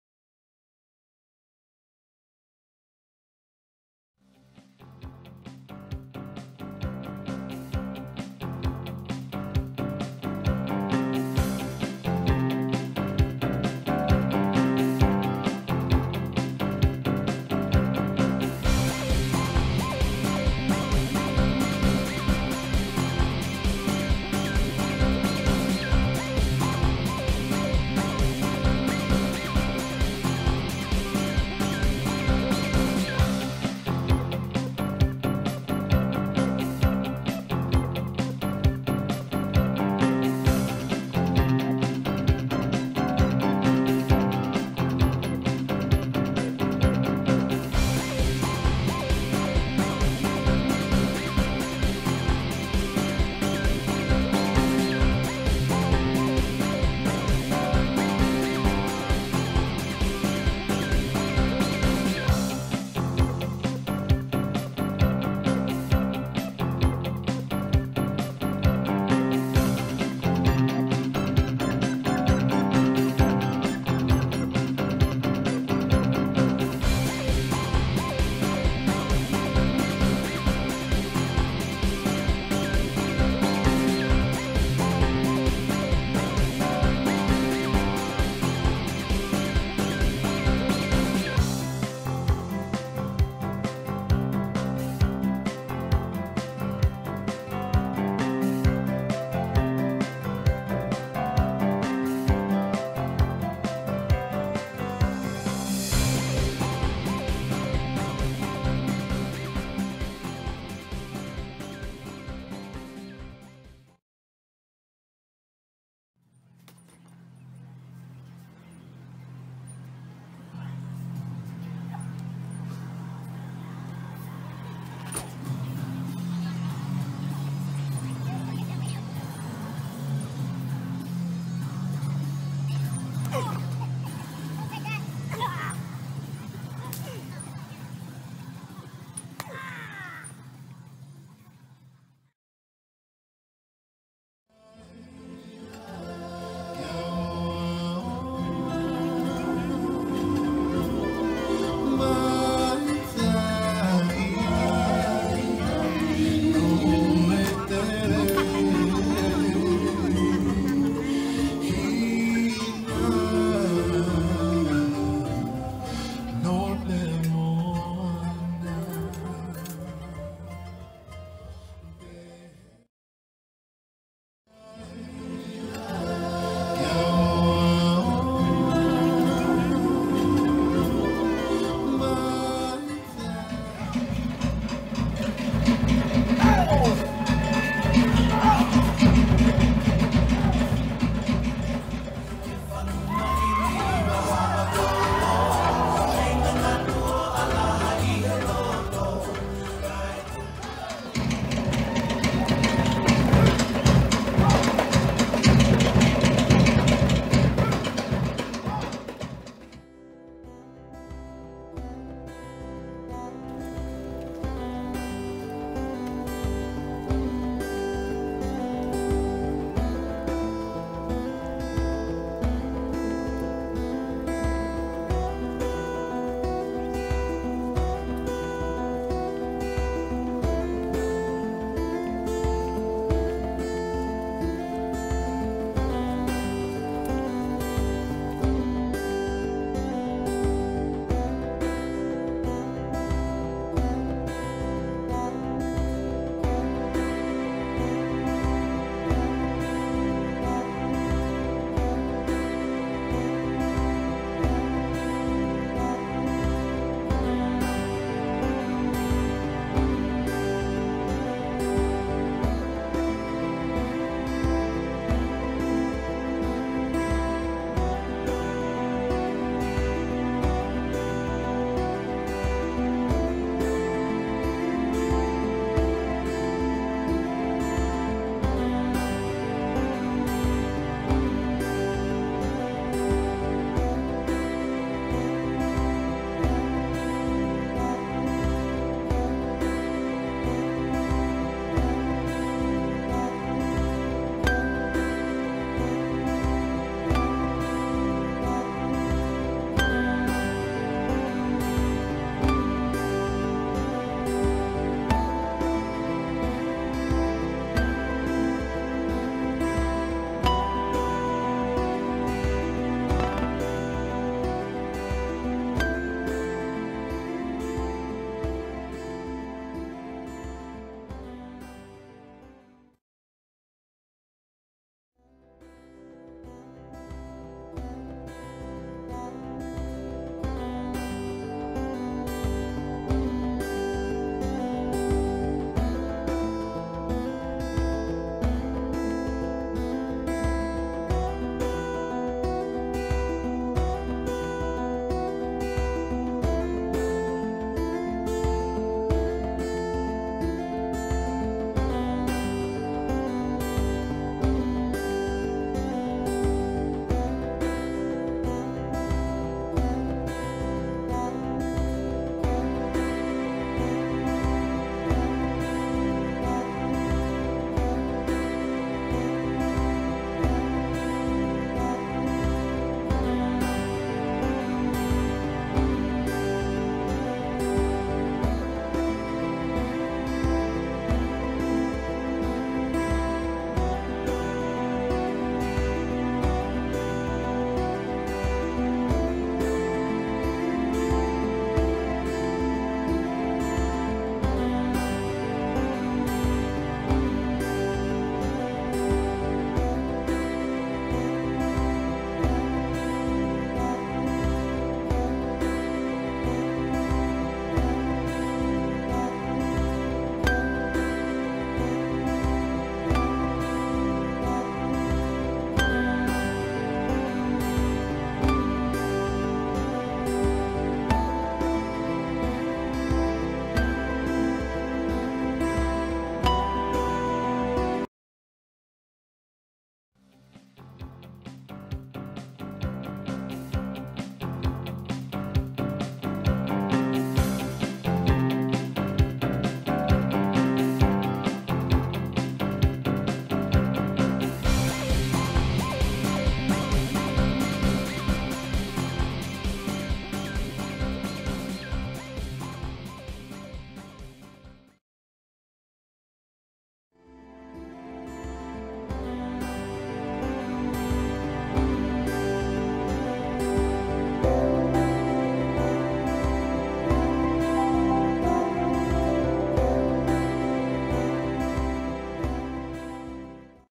Holiday in Waikiki, Hawaii
1 Week Holiday in Waikiki (2019 December, 3rd week)
Hotel: Hilton Waikiki Village
Flight: ANA 181, 182 (New A380)